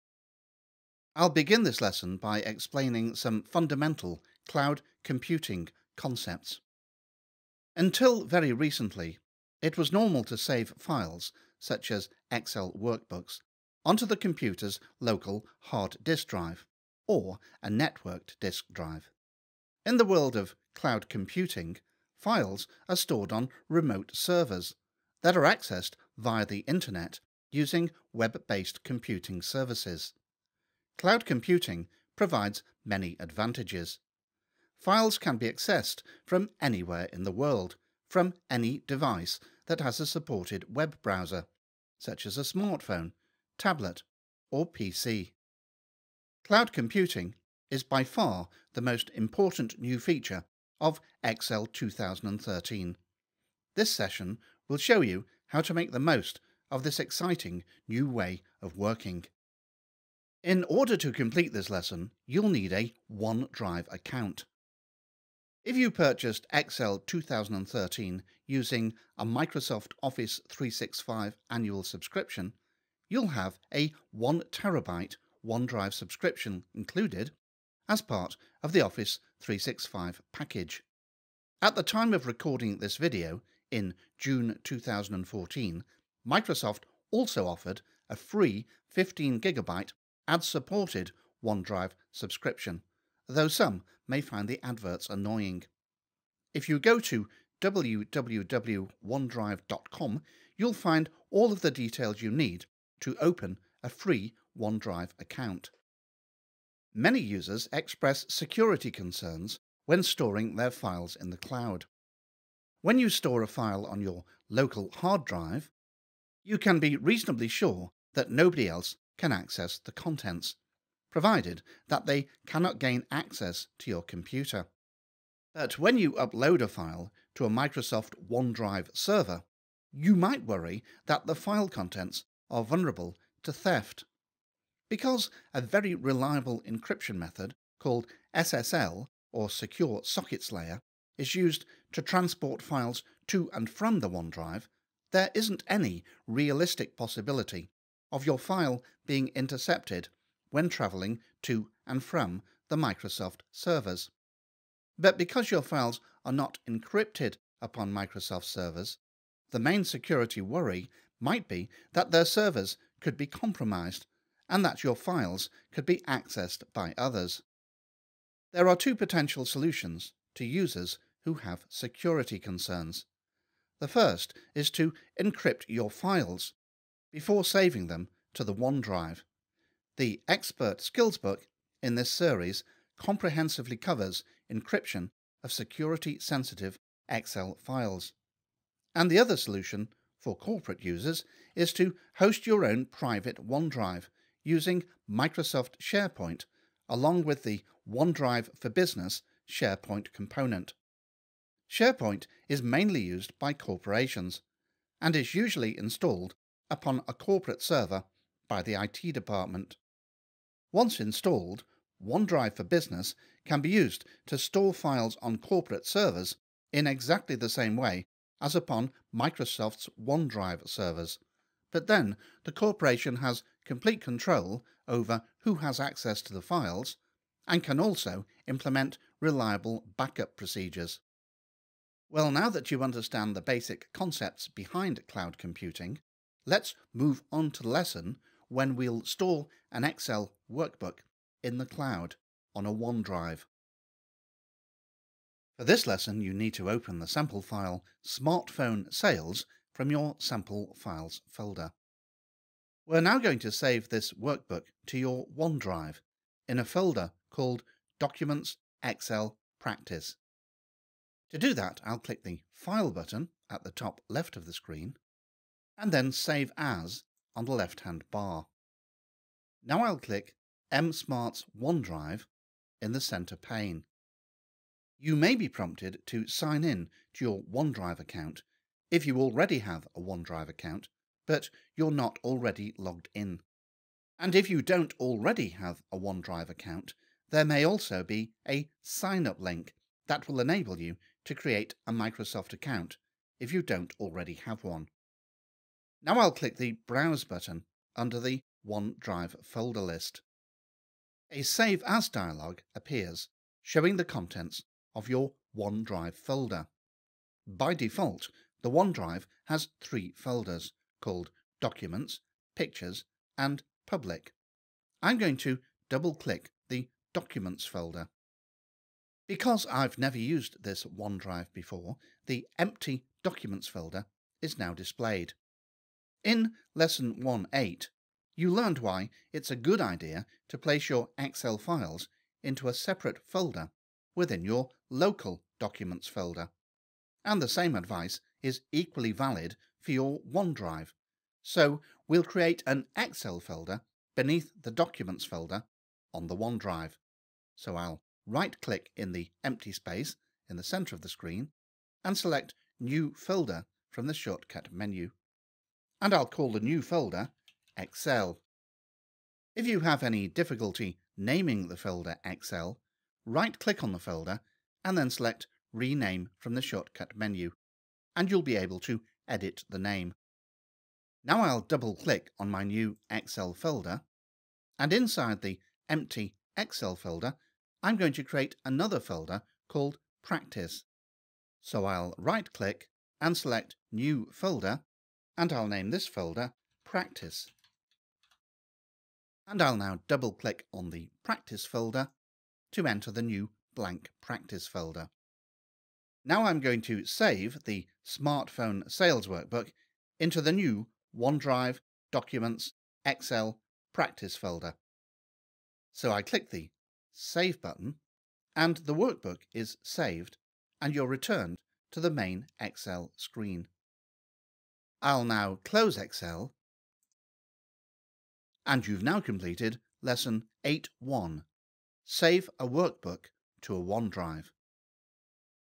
8-1: Save an Excel Workbook to a OneDrive
How to Save an Excel workbook to a OneDrive. A OneDrive can be visualized as a “hard drive in the sky”.  You can access OneDrive workbooks from any device, anywhere in the world.  You'll also gain an appreciation of cloud computing security issues.

This video comes from the "Cloud Computing" session (Session 8 in our YouTube Excel Essential Skills free video training course).